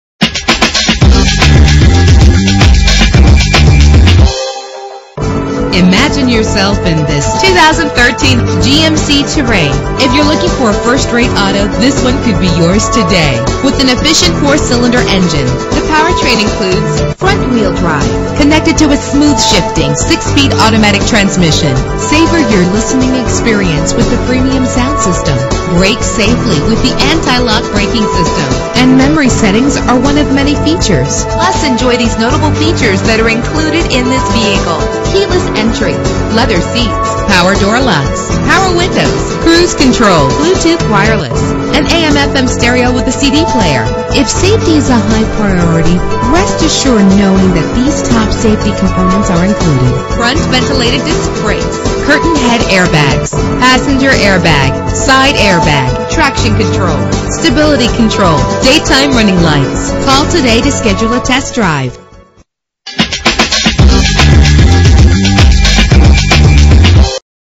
2013 GMC Terrain - White Bear Lake MN
Year: 2013
Make: GMC
Model: Terrain
Trim: SLT-1
Engine: 2.4 liter inline 4 cylinder 16 valve
Transmission: 6-Speed Automatic
Color: Crystal Red Tintcoat
Mileage:
Address:
3900 North Hwy 61